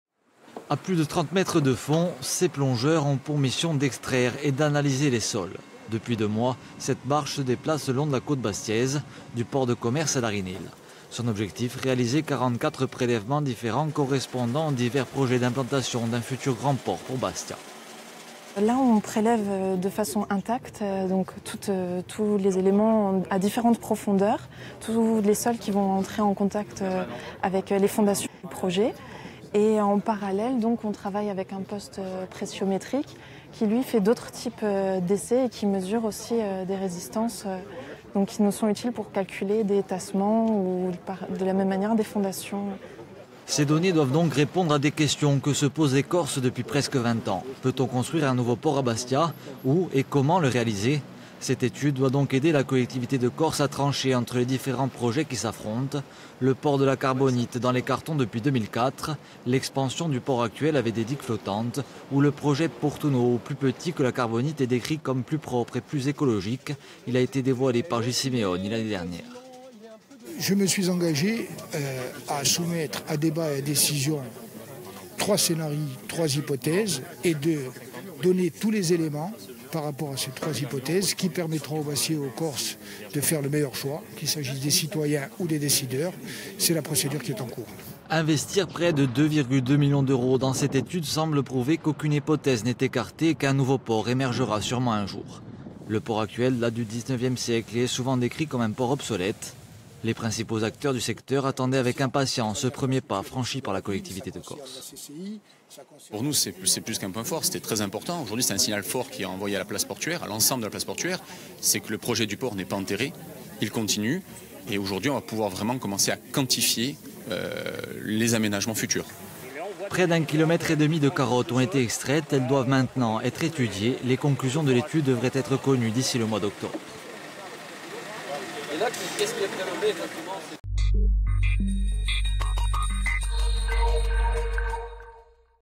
Fin des études pour choisir le nouveau port de Bastia
Intervenants -
Alison Schneider, ingénieur environnement et milieux marins
Gilles Simeoni, président du conseil exécutif de Corse
Stefanu Venturini, président de la commission du port de Bastia et vice-président de la Chambre de commerce et d’industrie (CCI) de Haute-Corse

Equipe -
F-A. Bernardi
G. Leonetti
J-P. Valentini
M. Maitrel